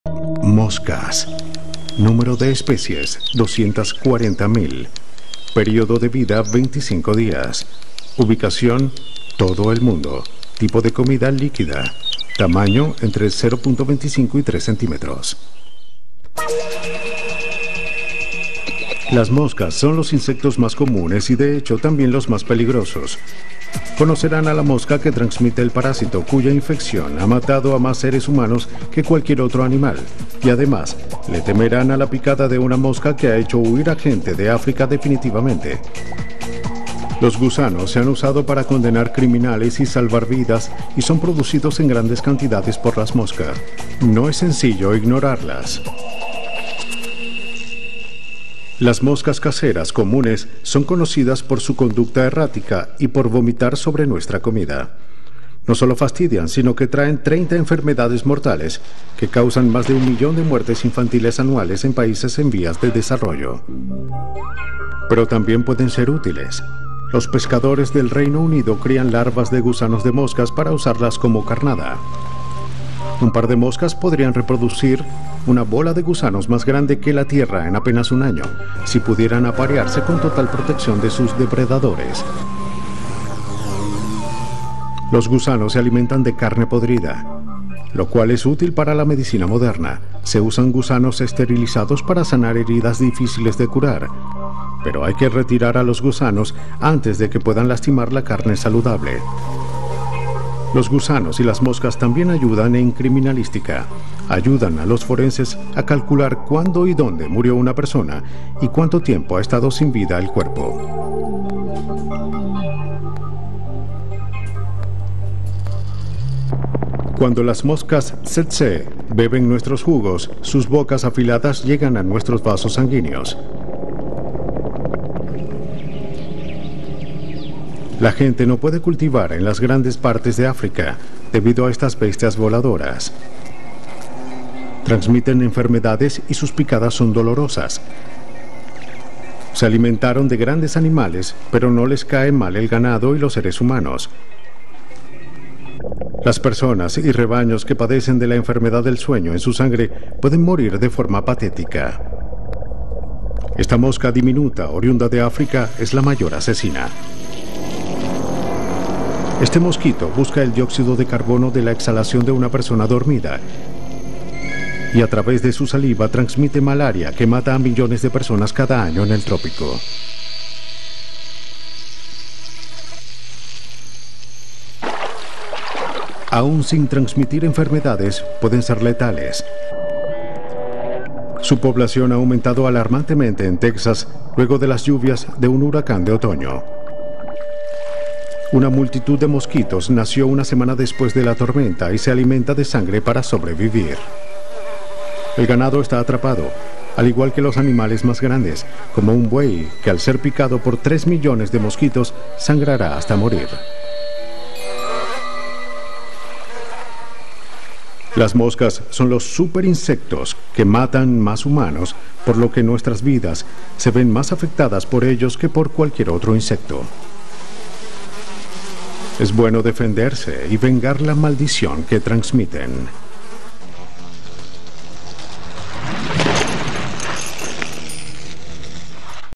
Las Moscas - Insectos Comunes
Al ser la mosca casera la más común de todas las especies y clases de mosca, ésta habita en todos los lugares donde el hombre se encuentre, incluyendo todos los climas del planeta. De ahi deviene su nombre popular.